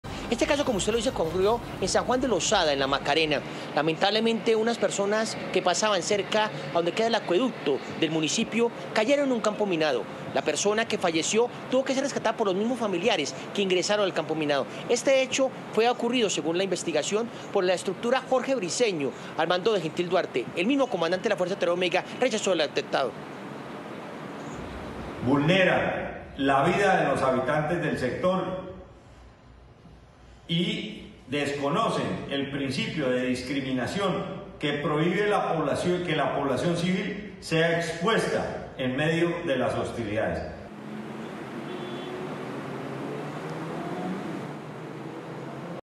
Un campesino muerto y otro herido tras caer en campo minado en zona rural de San Juan de Lozada
NoticiasRCN | El lamentable hecho se presentó zona rural de San Juan de Lozada, en la Serranía de la Macarena.